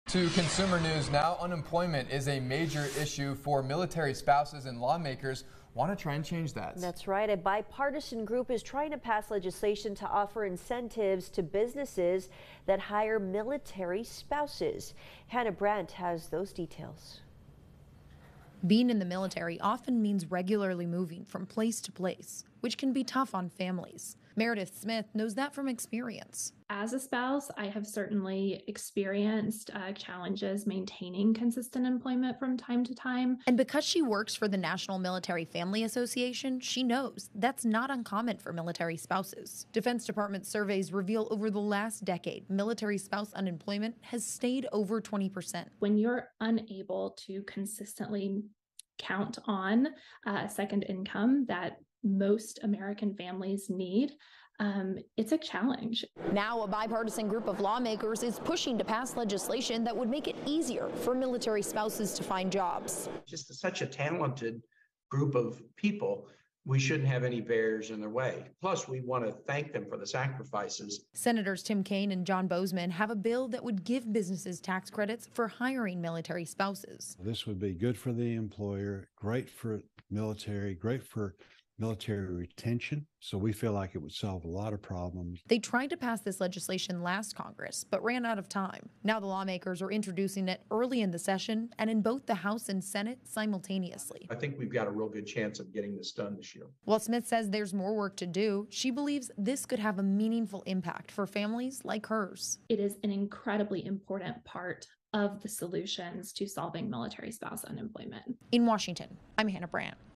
Consumer Alert: Employing military spouses
Lawmakers want to change unemployment issues among military spouses.